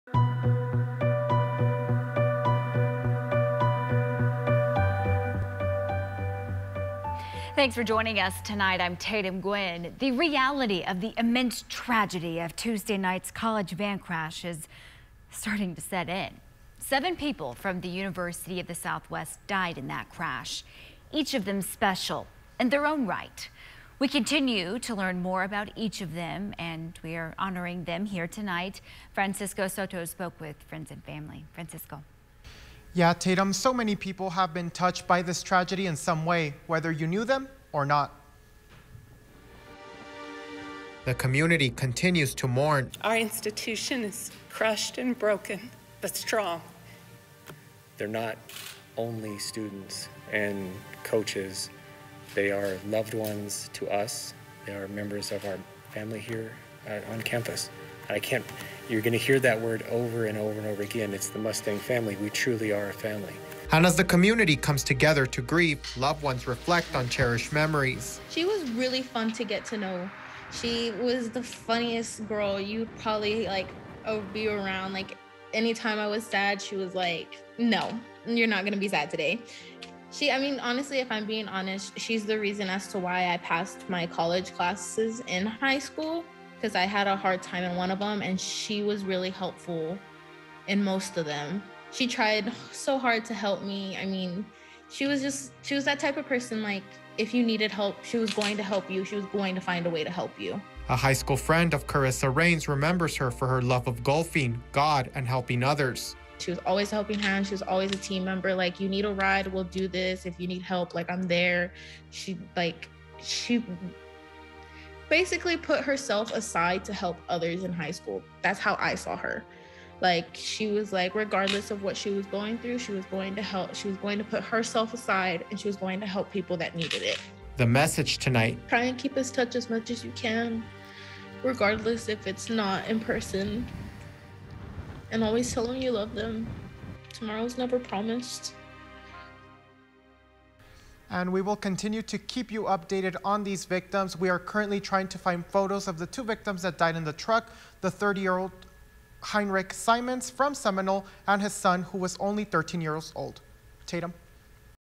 Friend of USW van crash victim reflects on fond memories
Annika Melendez, a high school friend of Karisa Raines, remembers her for her love of golfing, God and helping others.